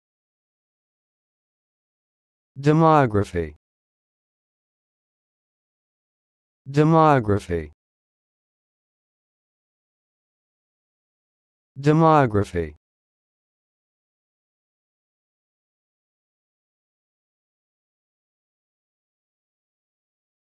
Learn how to say this word: "Demography"
This is the definition of "Demography":
n.  The study of races, as to births, marriages, mortality,
   health, etc.